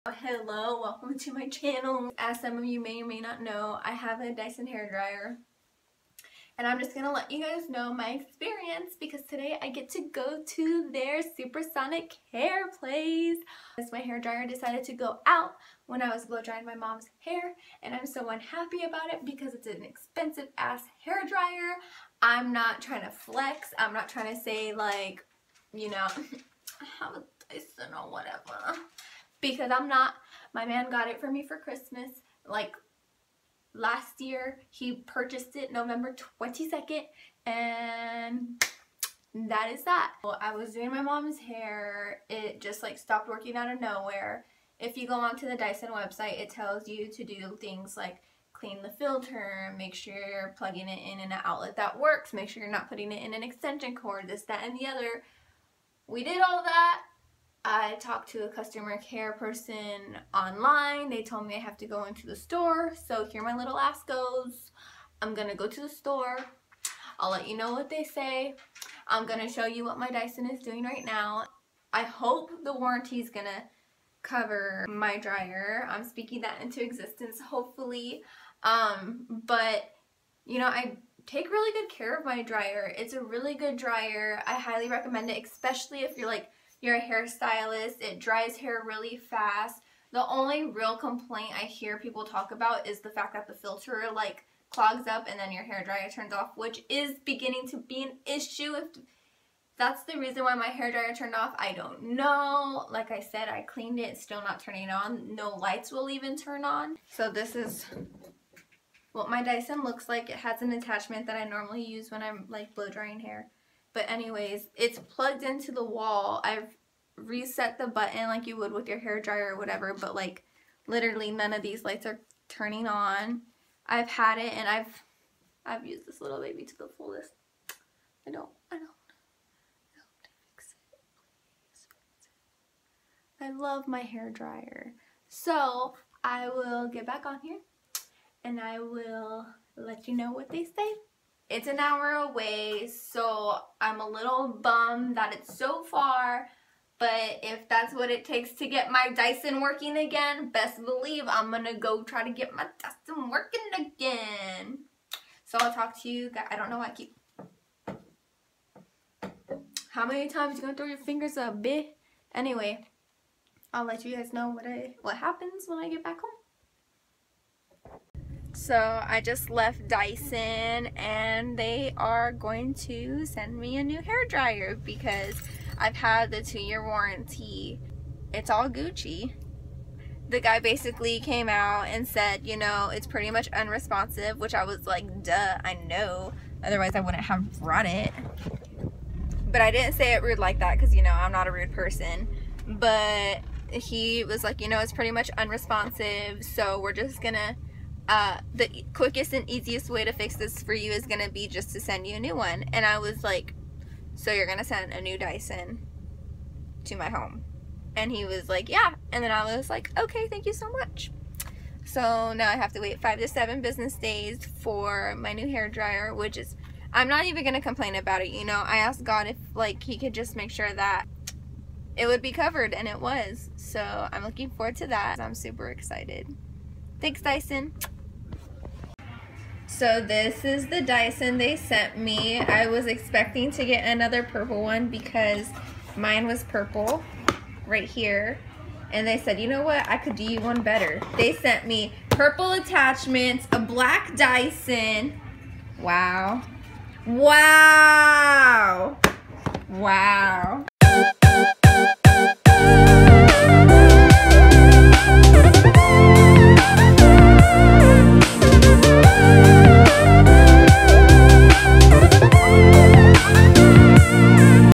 my dyson stopped working ! (review)
hello all!
here i explain what happened from the moment my dyson hairdryer turned off on me and what i did after!
please enjoy and share with friends who are thinking about getting a dyson dryer  or maybe a friend who has a dryer that shut off on them! they might have a 2 year warranty!